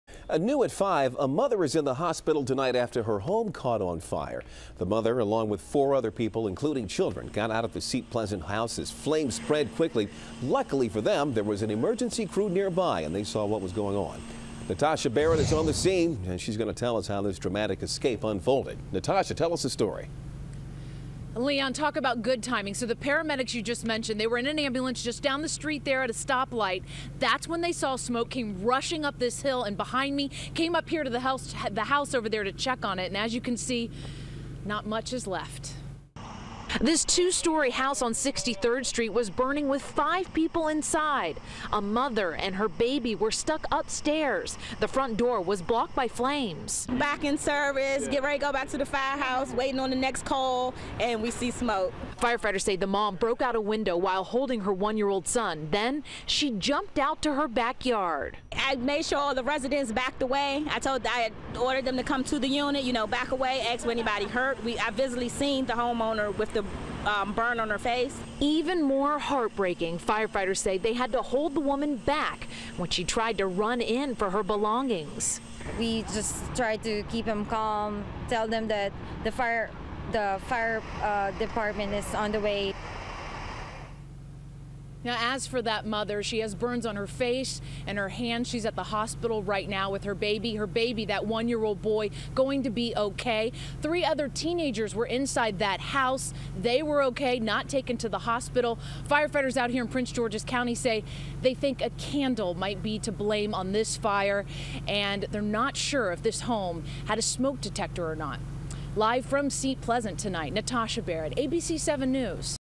Mother, three children escape Seat Pleasant fire
Paramedics happened to be just down the street at a stoplight, when they saw the smoke and came rushing up the hill to the house to check on it.